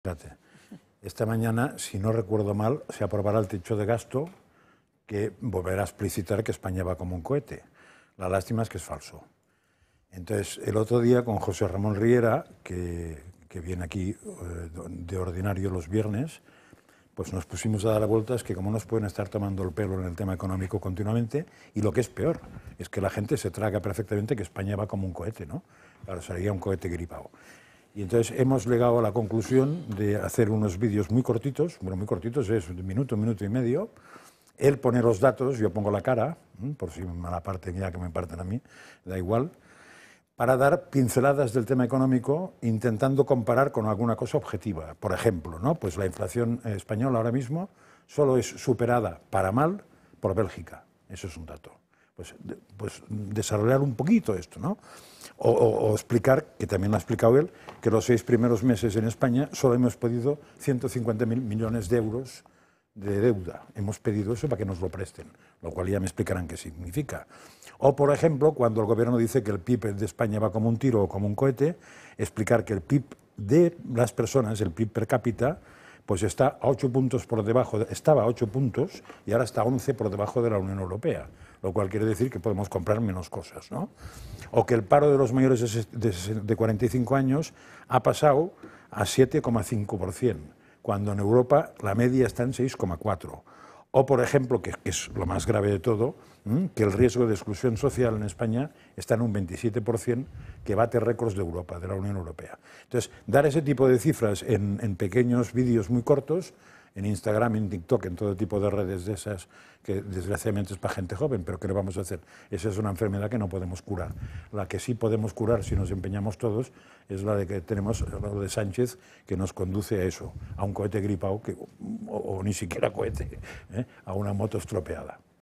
La enfermedad y las mentiras de Pedro Sánchez con el cohete de la economía
Josep Maria Francàs critica la aprobación del techo de gasto en España, argumentando que la afirmación de que el país "va como un cohete" es falsa.  Francàs menciona que la inflación en España es superada negativamente solo por Bélgica, que el país ha solicitado 150.000 millones de euros en deuda en seis meses, y que el PIB per cápita está 11 puntos por debajo de la media de la UE. Además, señala que el desempleo entre mayores de 45 años es del 7,5%, mientras que el riesgo de exclusión social alcanza el 27%, el más alto de la UE.